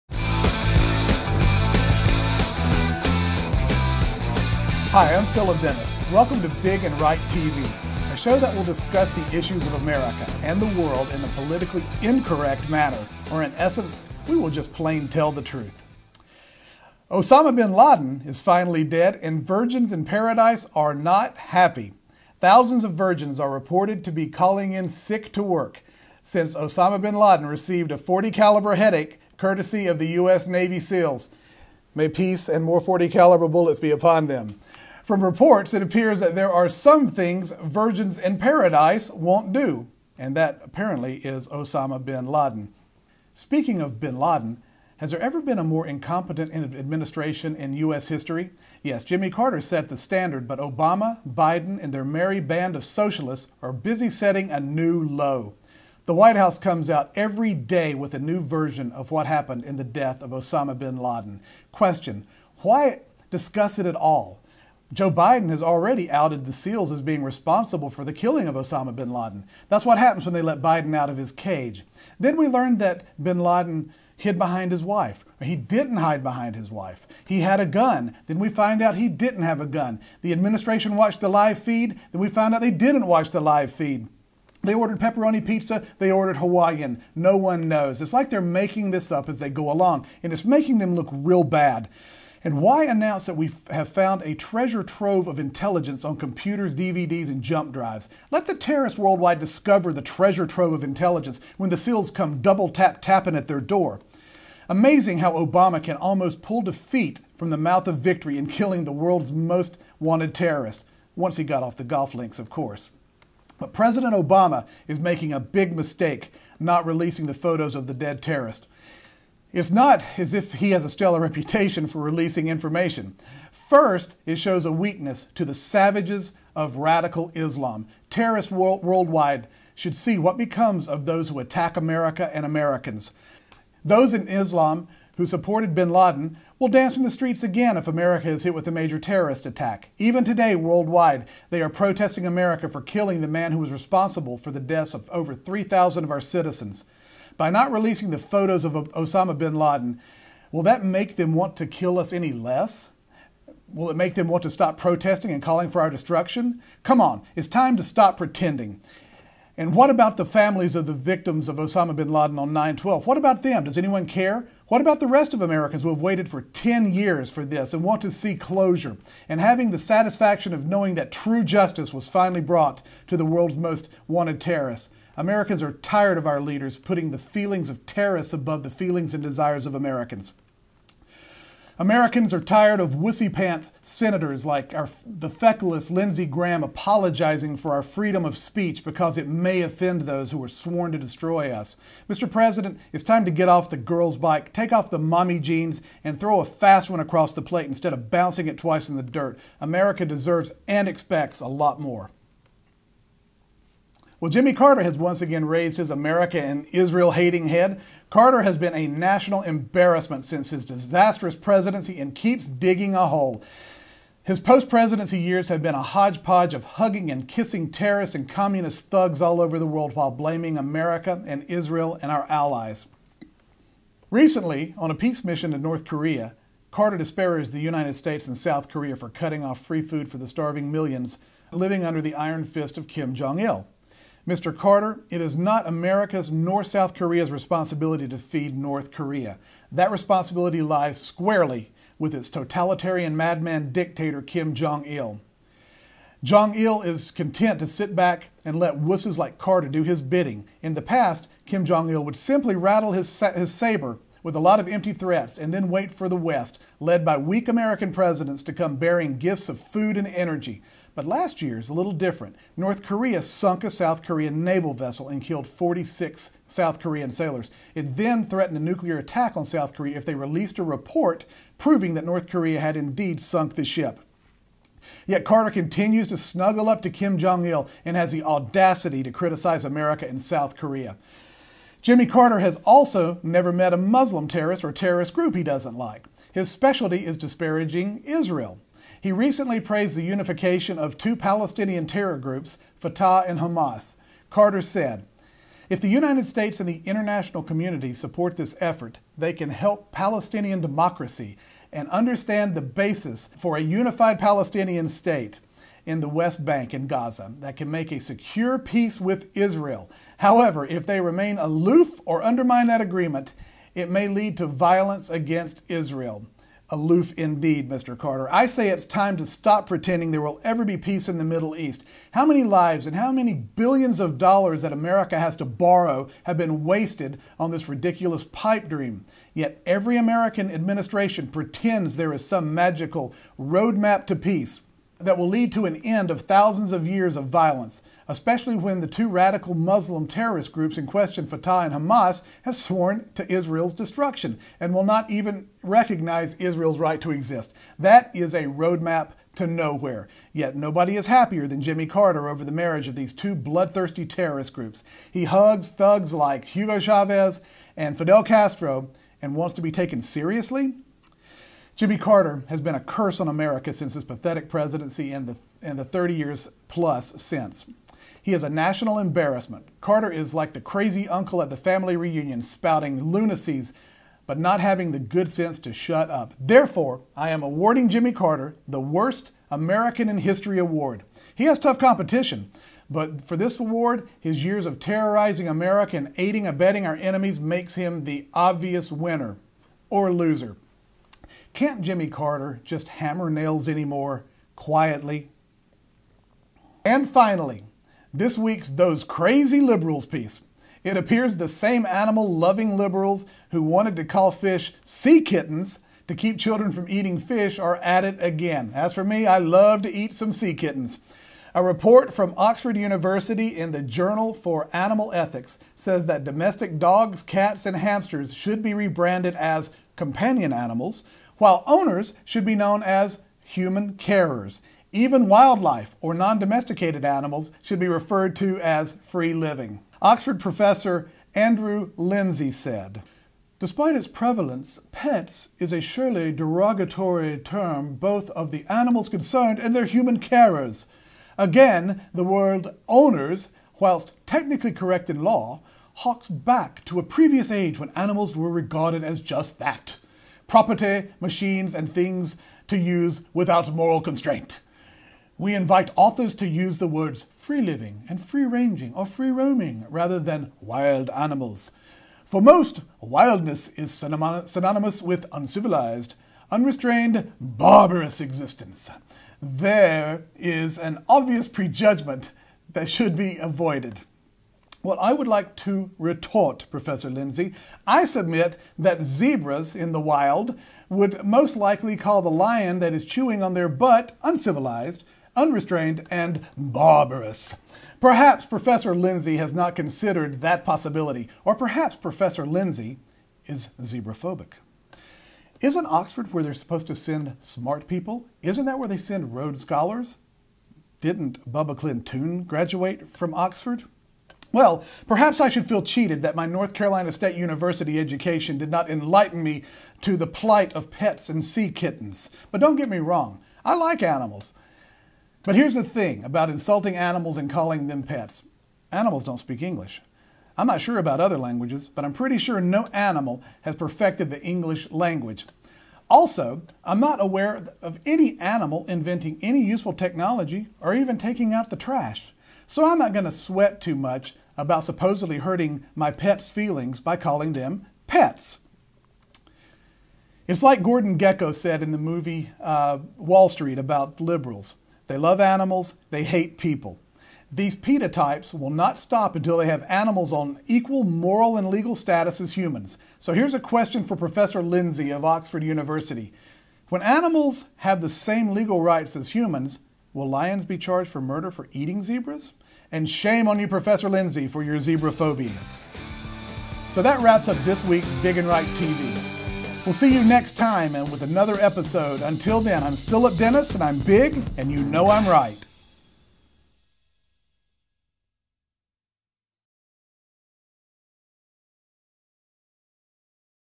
Big And Right - Episode # 1 Osama bin Laden, Carter and Sea Kittens
with Phillip Dennis

Osama bin Laden is finally dead and virgins in paradise are not happy. Thousands of virgins are reported to be calling in sick to to work sick since OBL received a .40 caliber headache courtesy of US Navy Seals (may peace and more bullets be upon them). From reports, it appears there are some things virgins in Paradise won't do. and OBL is one of them!...

Jimmy Carter has once again raised his America and Israel hating head! Carter has been a national embarrassment since his disastrous presidency and keeps digging a hole. His post presidency years have been a hodgepodge of hugging and kissing terrorist and communist thugs all over the world while blaming America and Israel...

It appears the same animal loving Libs who wanted to call fish "sea kittens" to keep children from eating fish are at it again! A report from Oxford University in the Journal for Animal Ethics says that domestic dogs, cats, and hamsters should be rebranded as "companion animals" while owners should be known as "human carers." Even wildlife, or non-domesticated animals, should be referred to as "free-living."...

Filmmaker for the Dallas Tea Party.